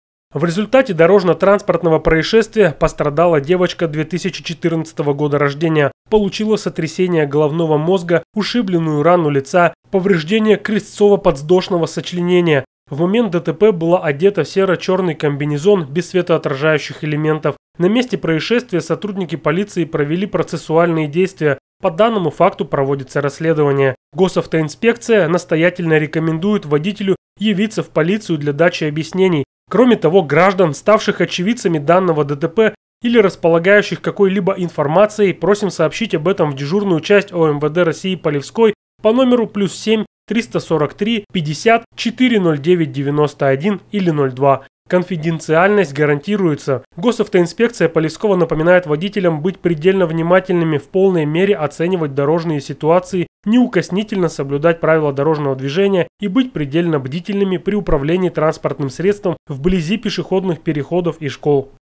Внимание, розыск!
Сегодня, около 8 часов утра на улице Розы Люксембург в районе дома 67 неустановленный водитель на автомобиле при движении в прямом направлении на пешеходном переходе сбил десятилетнюю  девочку. В настоящий момент водитель разыскивается сотрудниками полиции.